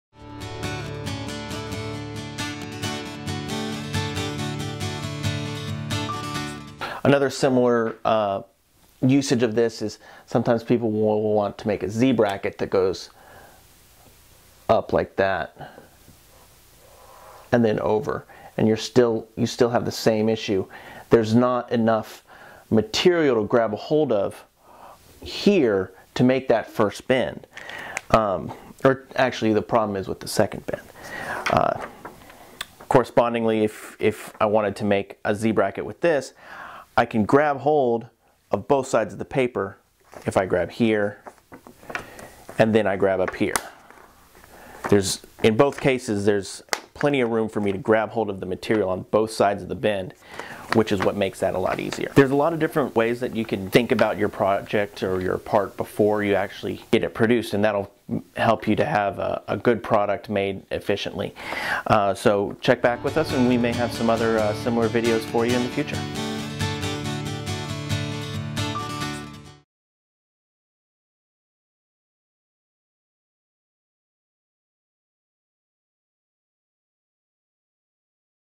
Quick Tips on Bracket Design - Part Two
A Short Run Pro Quick Tip Series - Difficulty Checks

This quick tip will show you how to design a Z-Bracket using a difficulty check with simply a piece of paper!